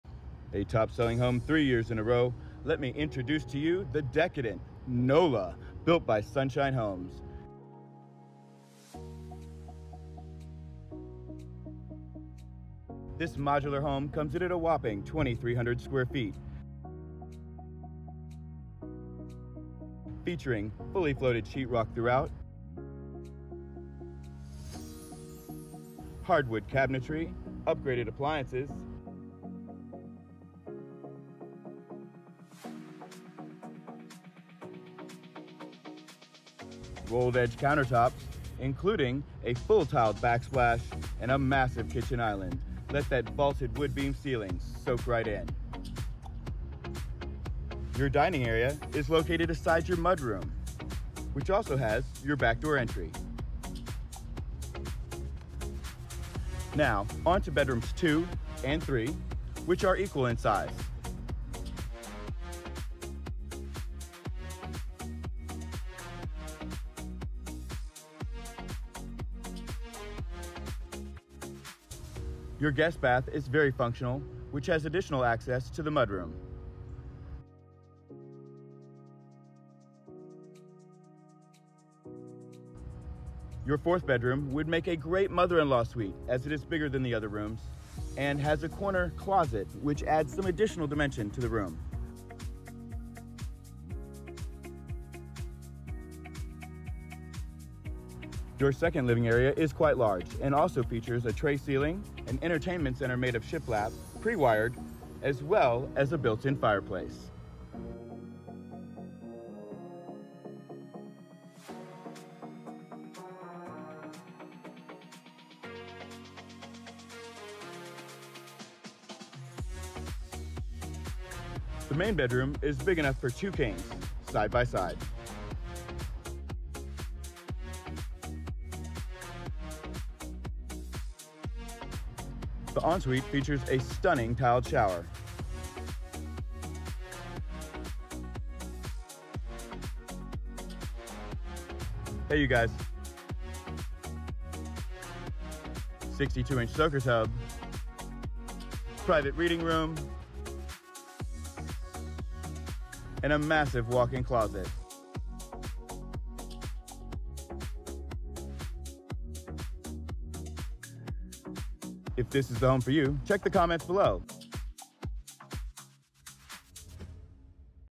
The "Nola" by Sunshine Homes
4 Bed / 2 Bath • 32' x 80' • 2,400 sqft.

This home is available for sale now at any Regional Homes or Town and Country Homes retailer. Interested in this home?